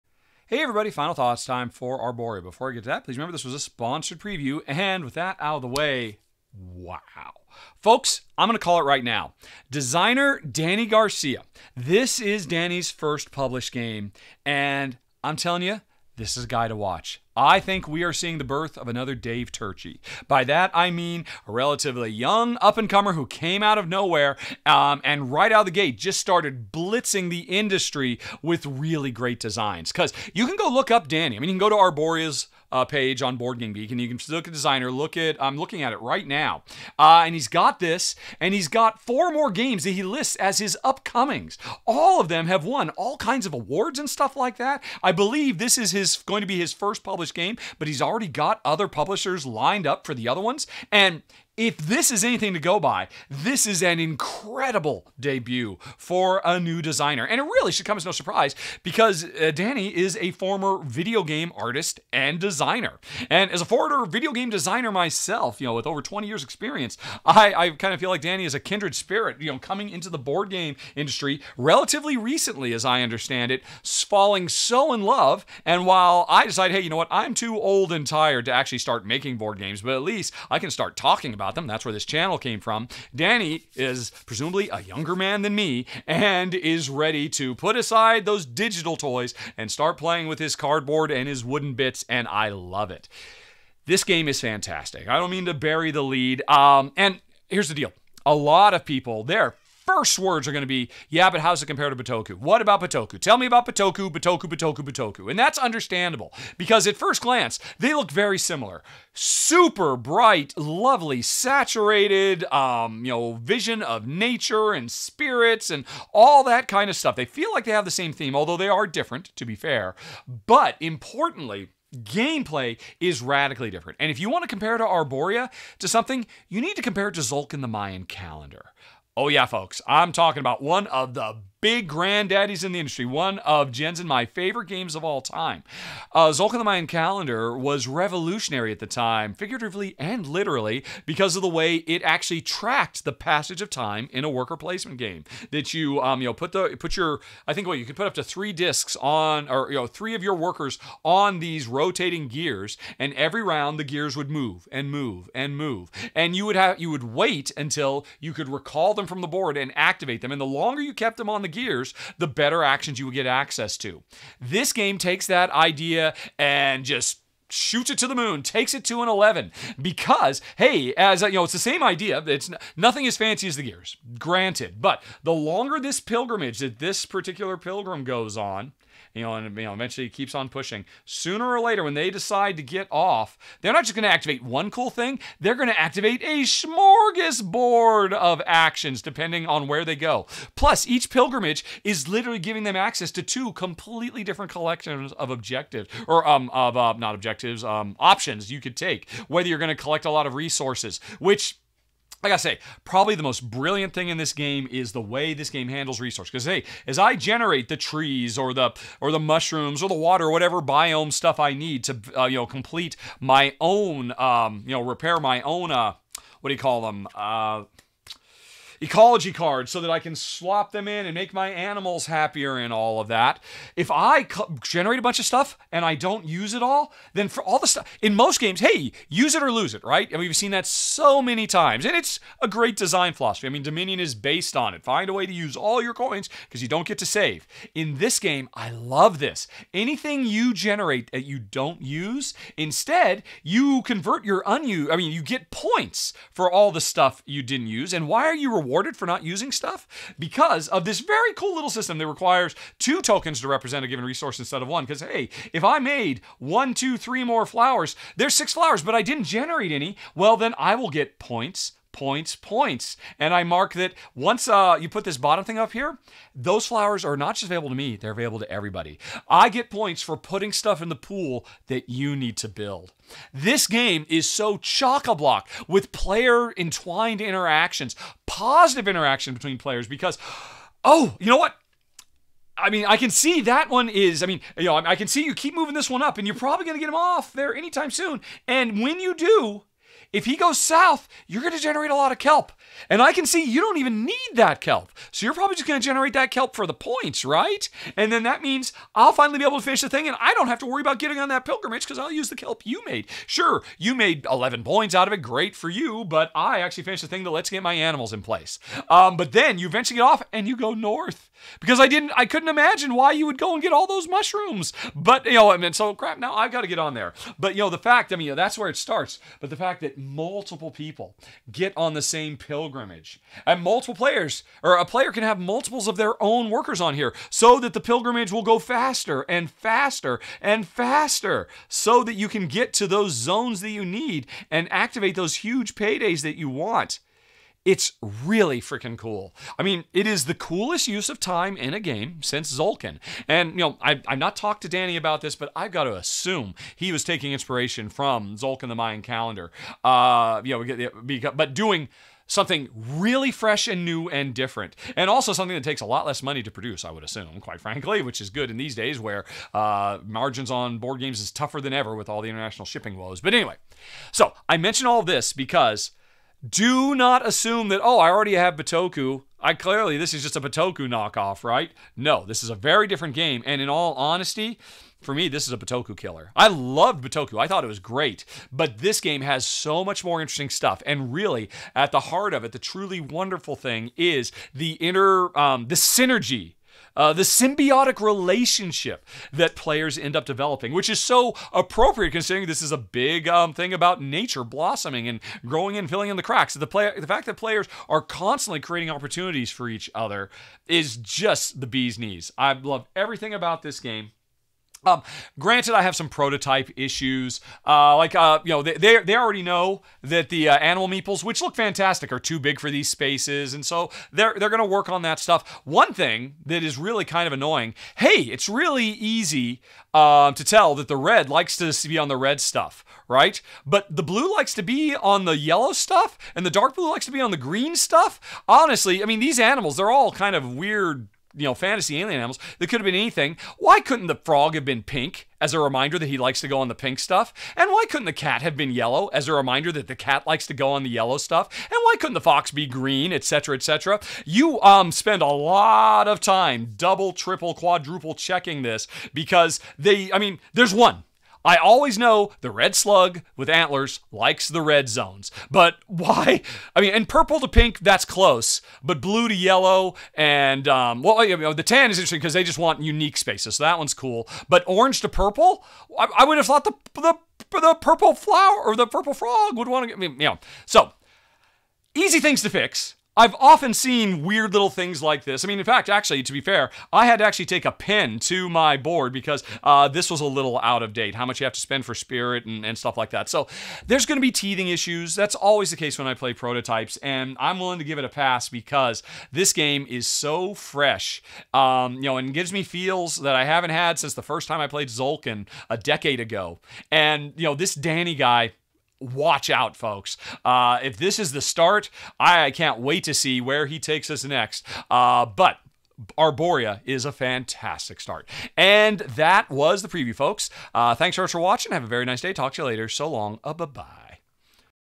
Arborea | Rahdo's Prototype Thoughts
A video outlining gameplay for the boardgame Arborea.